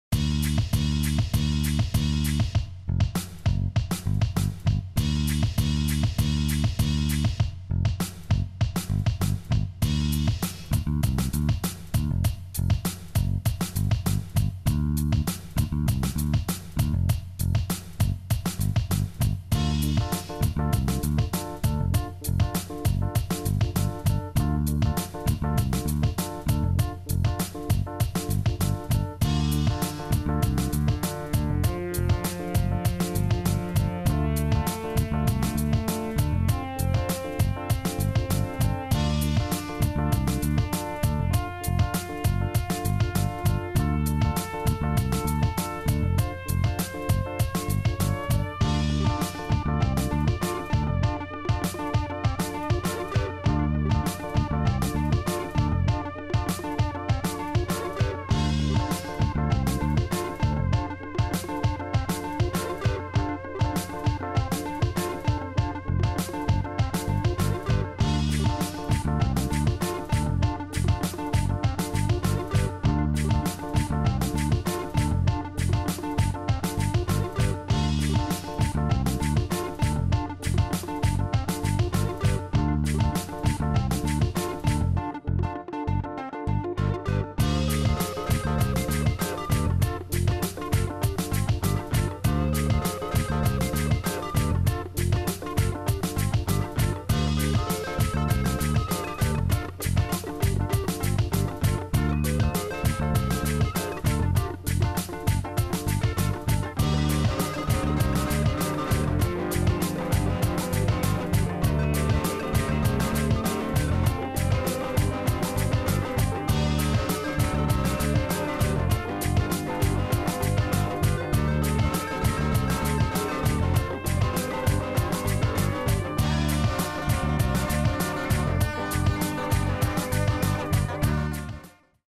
Timelapse Oil Painting Wassily Kandinsky "Autumn Landscape with Boats"
Copying "Autumn Landscape with Boats" by Wassily Kandinsky. I hope you enjoy the video.